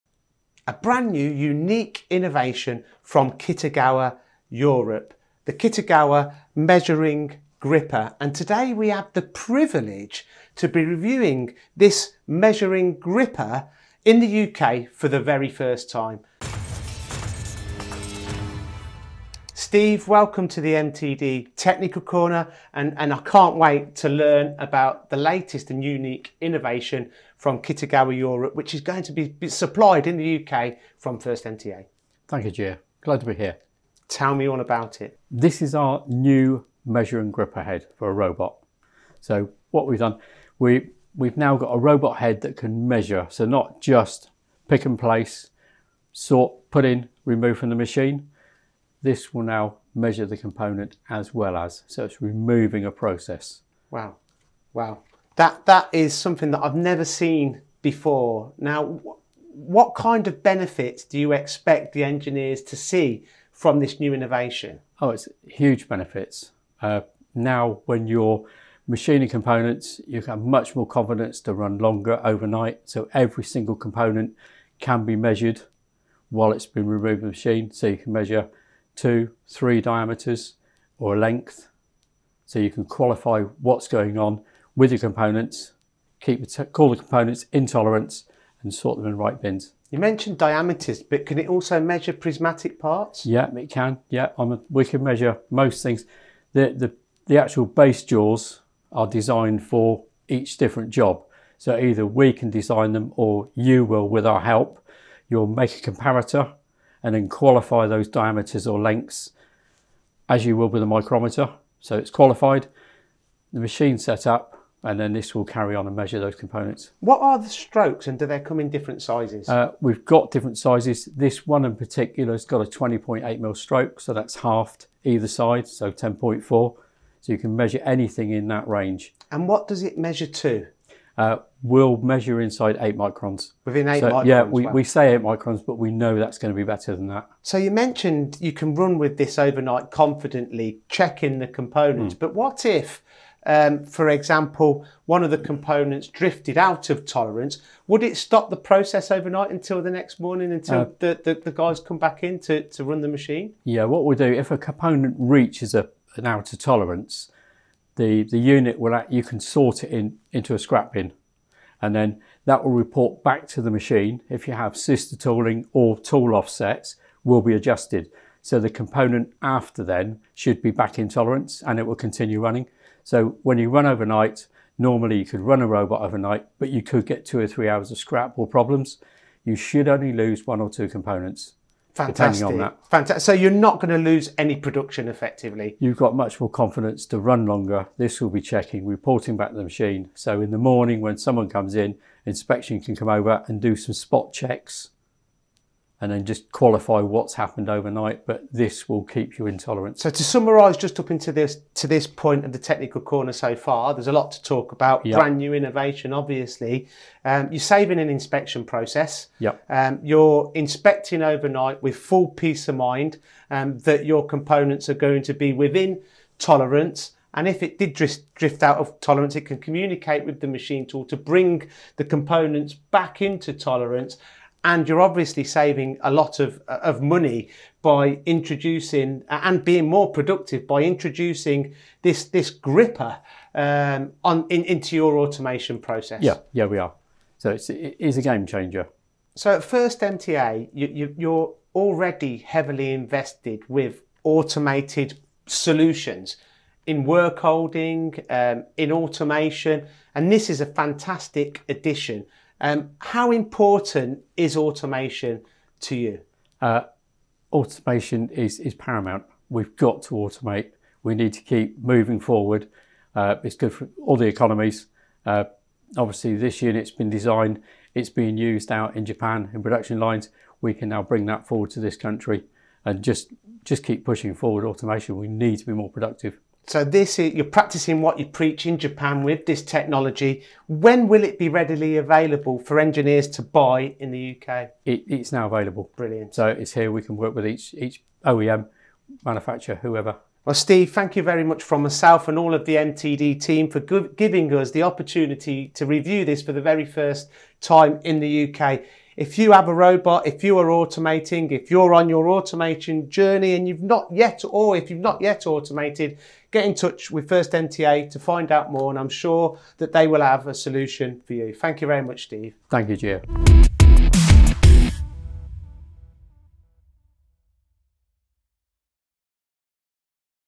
Have you ever seen a robot gripper head that can measure?
This new robot head from 1st Machine Tool Accessories can do much more than pick and place, sort, and remove – it can measure the component as well!

Engineers are going to see some massive benefits from this innovation, and Steve Ellicott of 1st Machine Tool Accessories tells us how.

If you want increased confidence to run longer overnight, check out this new technology that lets you qualify what’s going on with components, call them in tolerance, and sort them in the right bins!